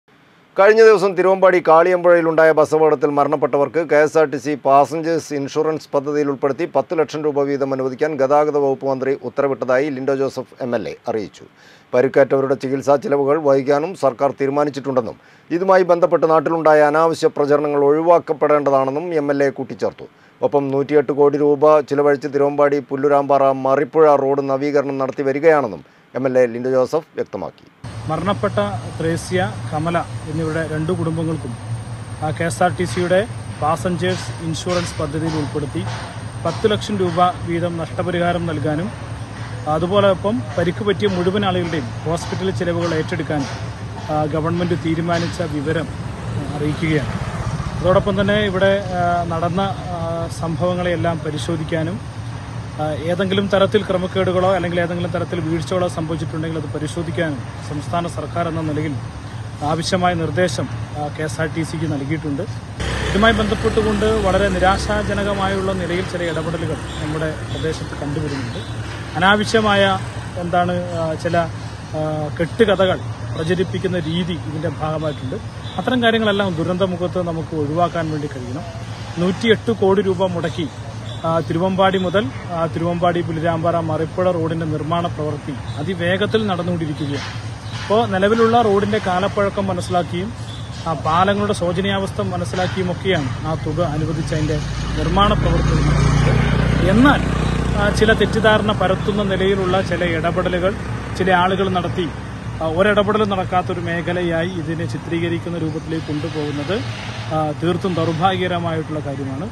കഴിഞ്ഞ ദിവസം തിരുവമ്പാടി കാളിയമ്പുഴയിൽ ഉണ്ടായ ബസ്സപകടത്തിൽ മരണപ്പെട്ടവർക്ക്
കഴിഞ്ഞ ദിവസം തിരുവമ്പാടി കാളിയമ്പുഴയിൽ ഉണ്ടായ ബസ്സപകടത്തിൽ മരണപ്പെട്ടവർക്ക് കെ.എസ്.ആർ.ടി.സി പാസ്സഞ്ചേഴ്‌സ് ഇൻഷുറൻസ് പദ്ധതിയിൽ ഉൾപ്പെടുത്തി പത്ത് ലക്ഷം രൂപ വീതം അനുവദിക്കാൻ ഗതാഗതവകുപ്പ് മന്ത്രി ഉത്തരവിട്ടതായി ലിന്റോ ജോസഫ് എം.എൽ.എ . അറിയിച്ചു.